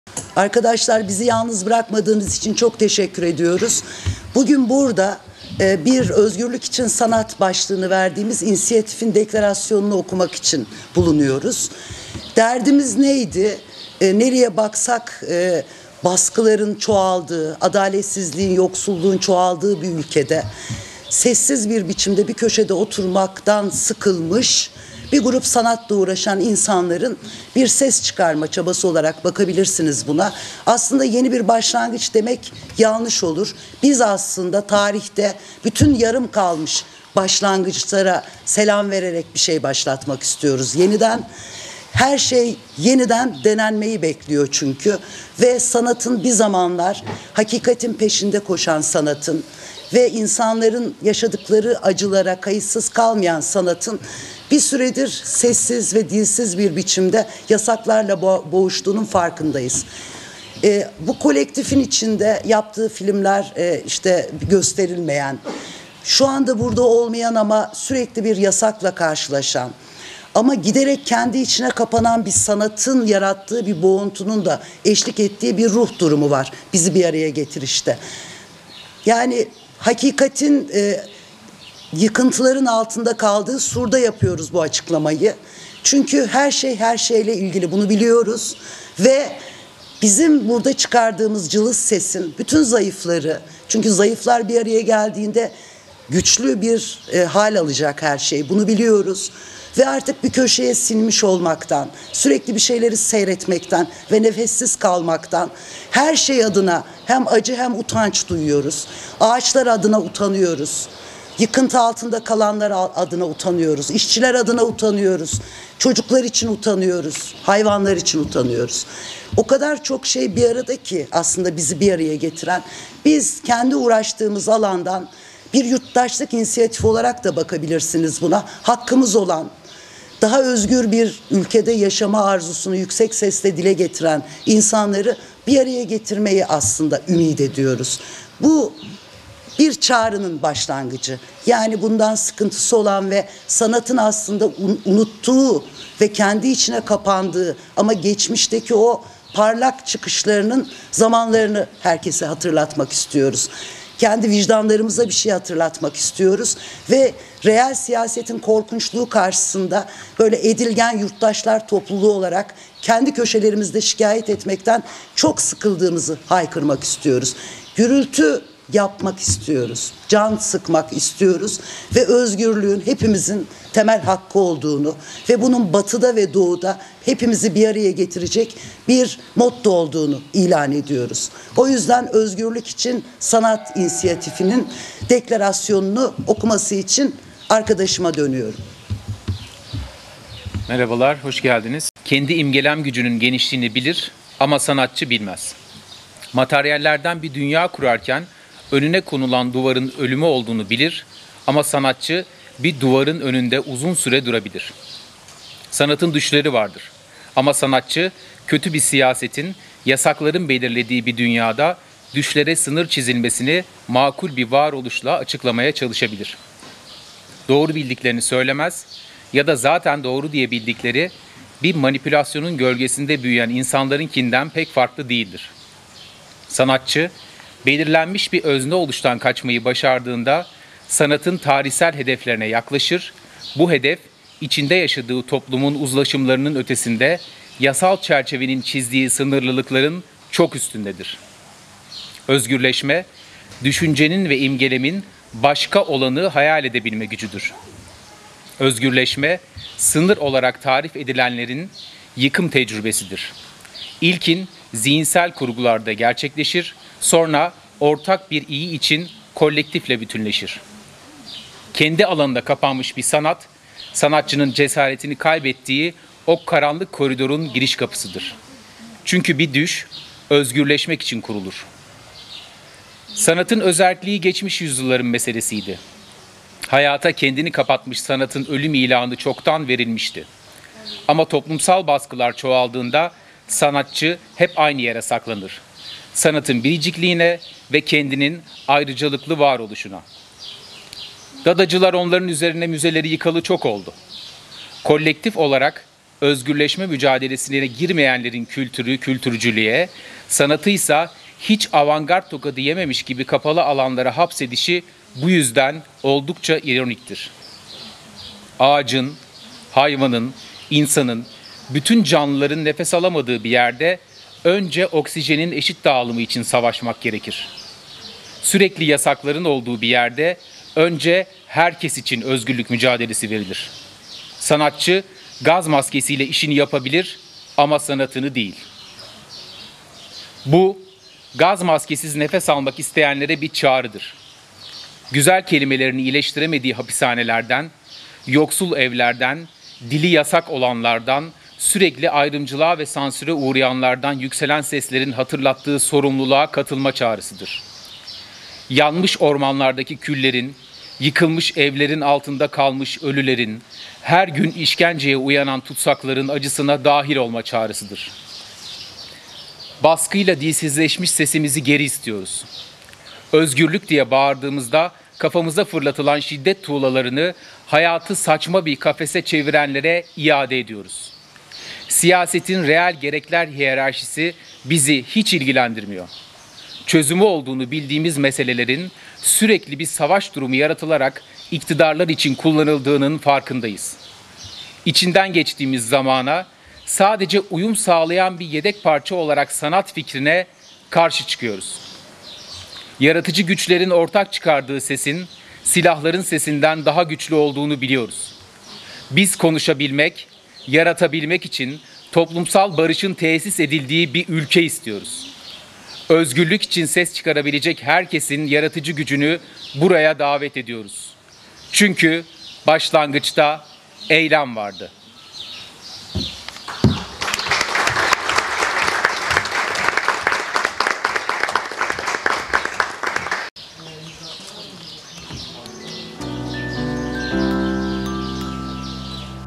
ÖZGÜRLÜK İÇİN SANAT İNİSİYATİFİ KURULDU: BARIŞIN TESİS EDİLDİĞİ BİR ÜLKE İSTİYORUZ
Özgürlük İçin Sanat İnisiyatifi’nin kuruluşu deklere edildi. Açıklamada, "Sürekli yasakların olduğu bir yerde önce özgürlük mücadelesi verilir. Biz konuşabilmek, yaratabilmek için toplumsal barışın tesis edildiği bir ülke istiyoruz" denildi